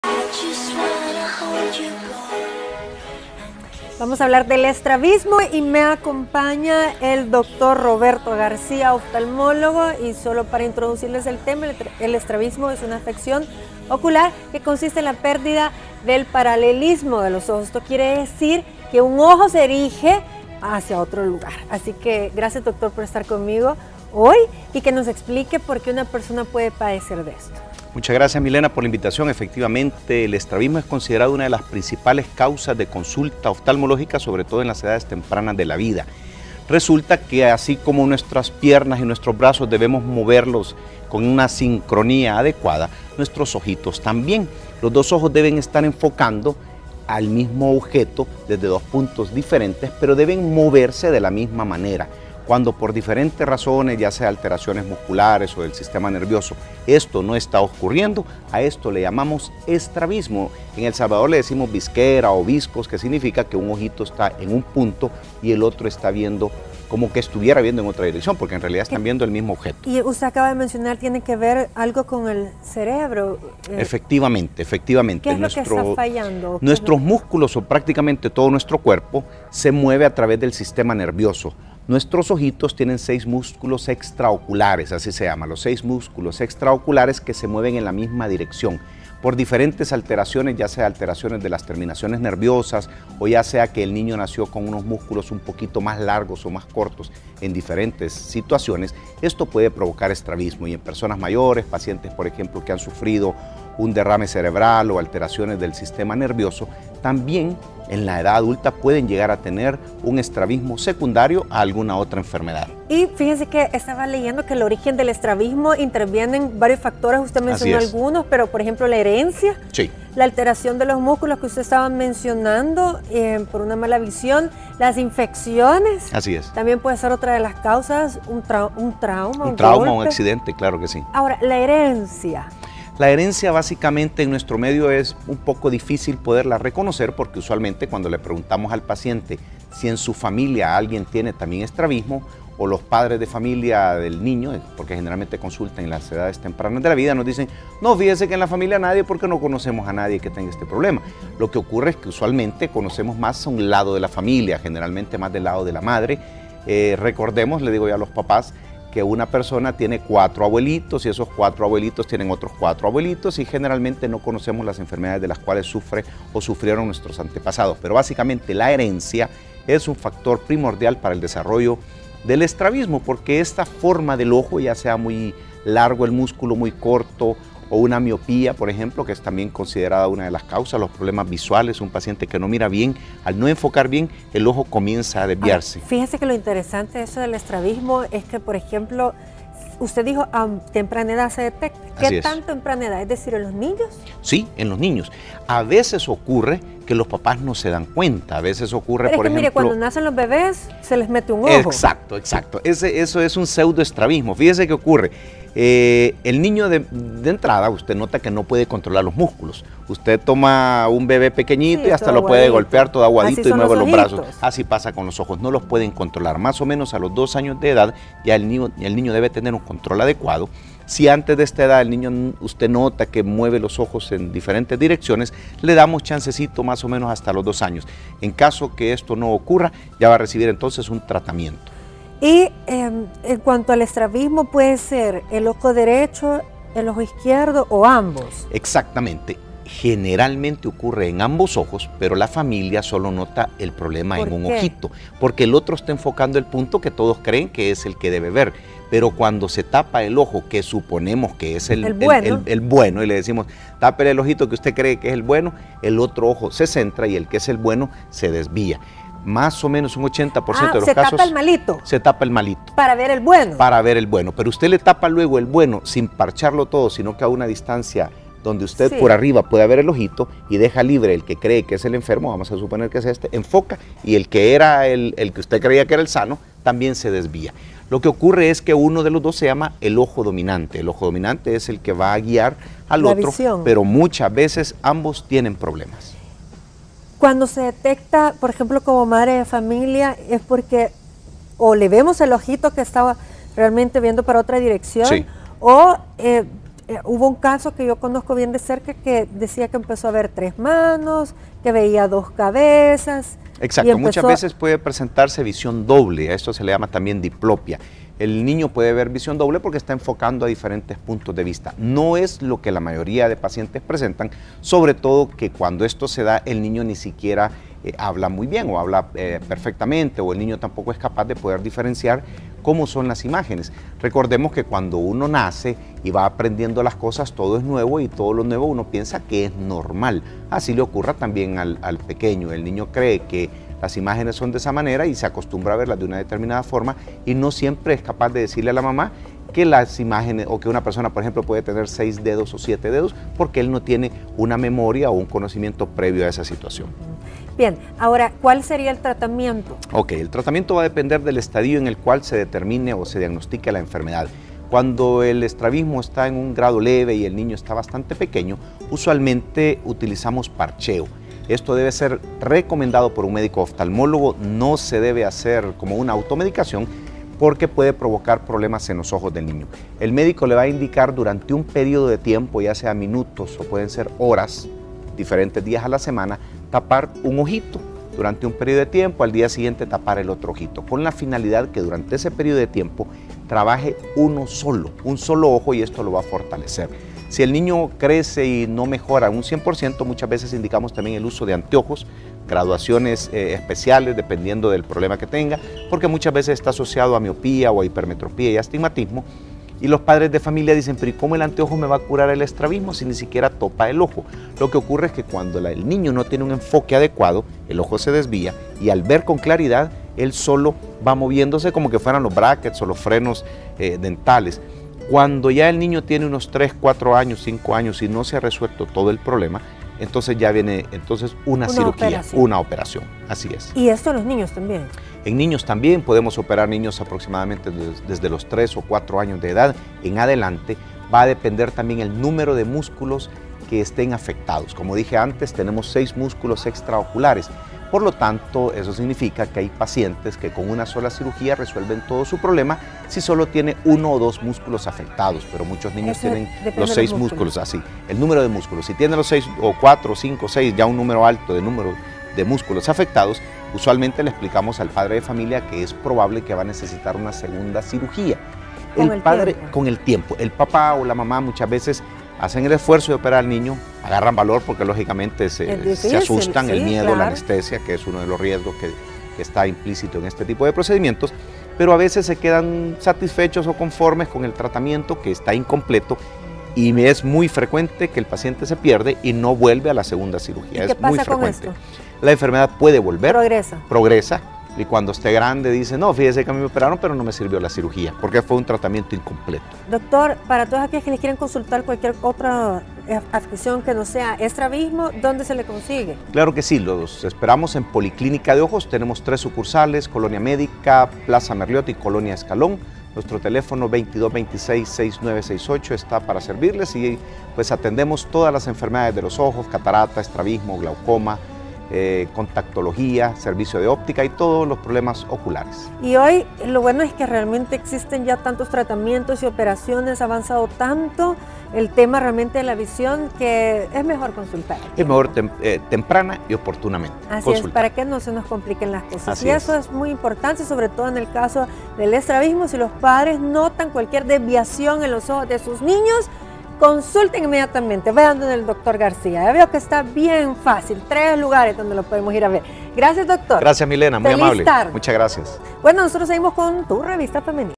El Estrabismo
Afección ocular que consiste en la pérdida del paralelismo de los ojos, es decir, que uno de ellos se erige hacia otro lugar. Causas y consecuencias